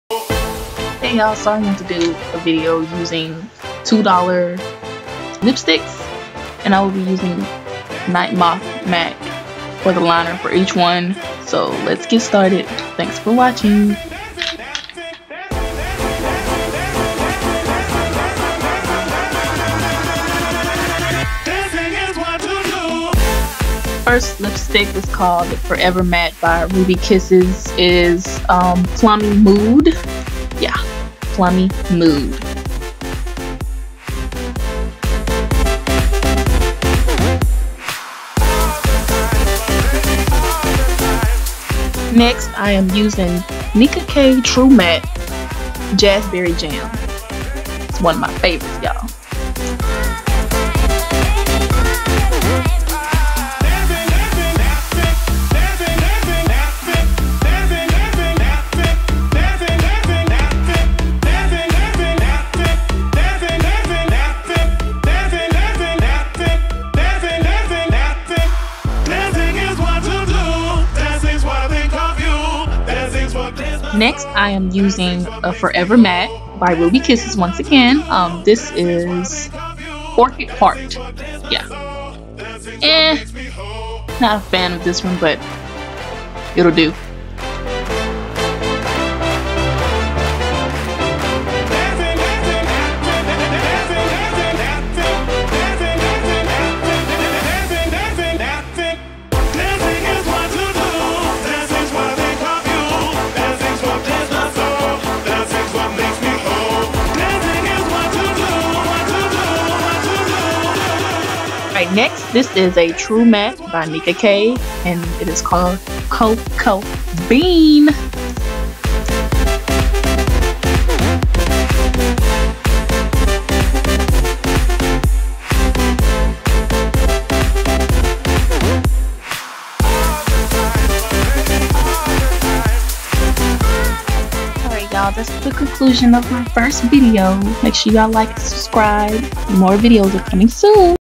$2 MATTE LIPSTICKS FOR WOC
PRODUCTS MENTIONED                                                                      First Lipstick: Ruby kisses Forever Matte  "Plummy Mood"                  Second Lipstick:  Nicka K True Matte  "Jazzberry Jam"
Third Lipstick: Ruby Kisses Forever Matte  "Orchid Heart"
Fourth Lipstick: Nicka K True Matte "Cocoa Bean"

**I OWN NO RIGHTS TO THE MUSIC USED IN THIS VIDEO